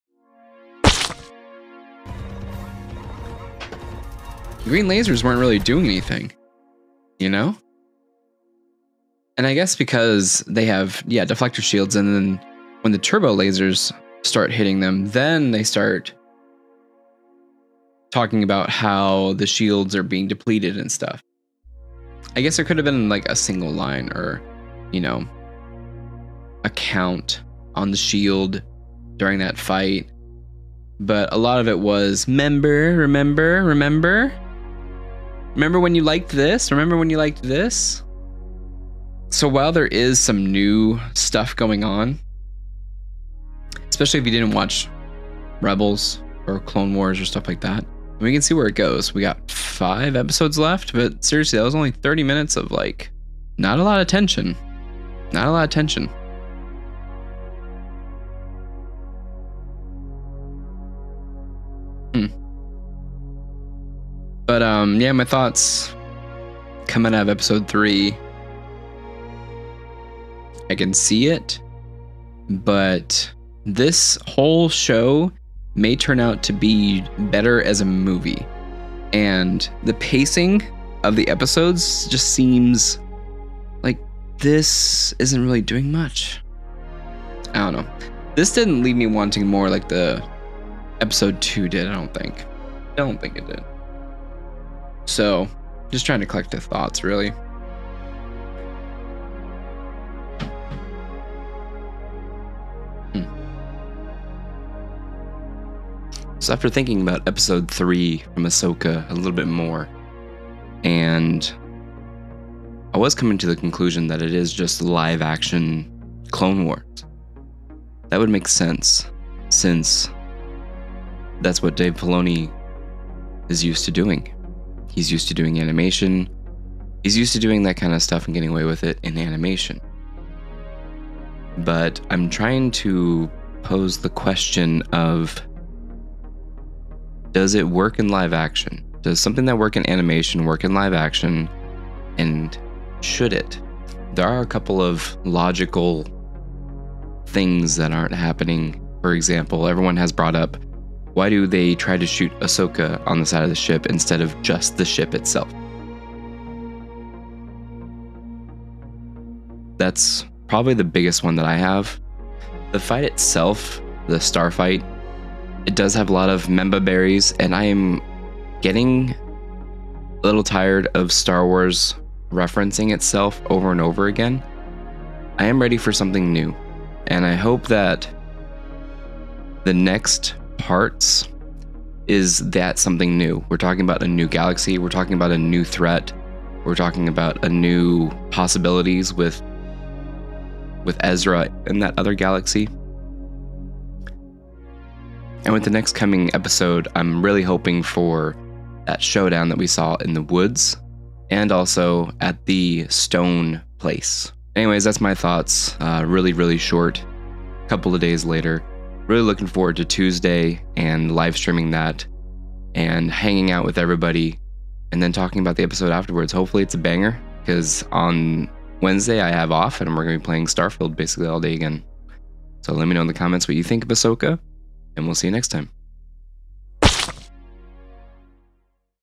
AHSOKA EPISODES 3 REVIEW - Does live action Clone Wars work?
Star Wars fans, how are you feeling after episode 3? This was streamed live on YouTube for a watch party and a couple of days later. Does what happens in Clone Wars and Rebels work in live action? Let me know your thoughts?

If you dig this, feel free to check out my thoughts on recent Marvel projects